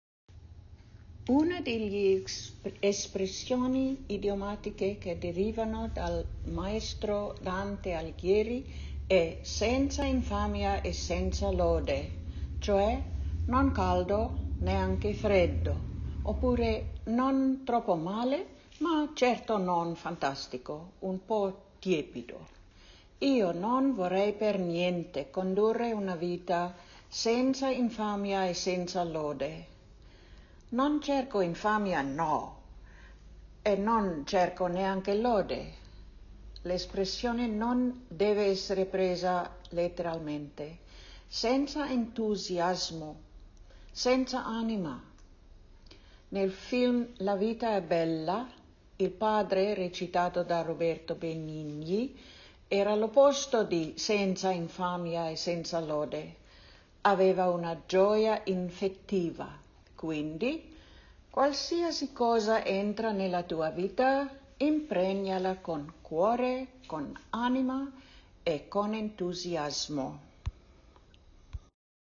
Ilse spiega il significato dell'espressione SENZA INFAMIA E SENZA LODE
As part of our Dante celebrations for the 700 Anniversary of his death, we asked our students to interpret the definition of some interesting words by Dante.